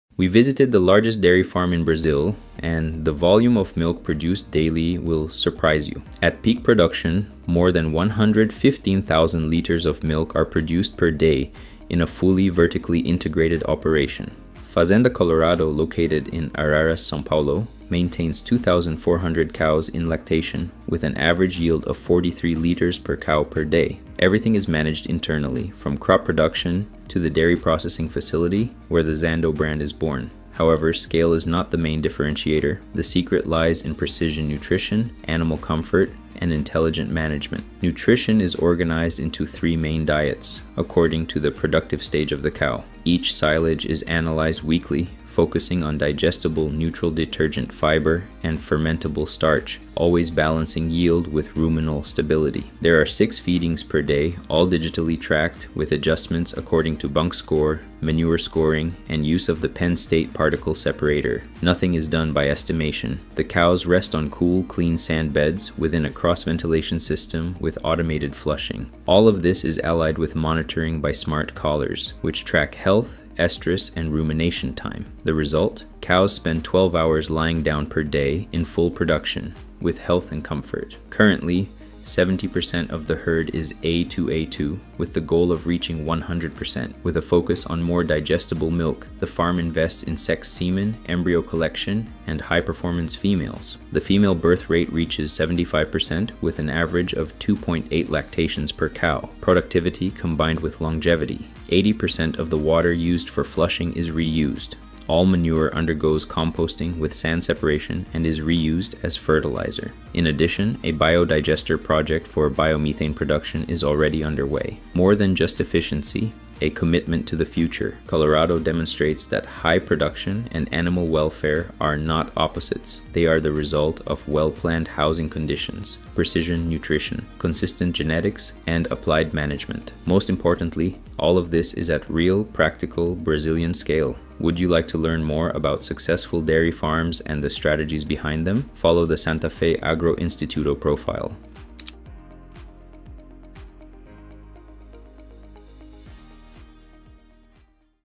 This farm in Brazil produces over 100,000 liters of milk per day.
High production is not a matter of luck—it is the result of management, nutrition, and welfare taken seriously. 🐄

At Fazenda Colorado, more than 100,000 liters of milk per day are produced based on precision nutrition, thermal comfort, A2A2 genetics, and total control of the feeding and reproductive routine.

A complete, vertically integrated system focused on efficiency, longevity, and sustainability. ✅

This is a real, Brazilian model and one from which we can learn.

Tag a producer or technician who needs to know what a true high-performance dairy farm is.

👉 Follow the Santafé Agroinstituto profile to receive more content like this.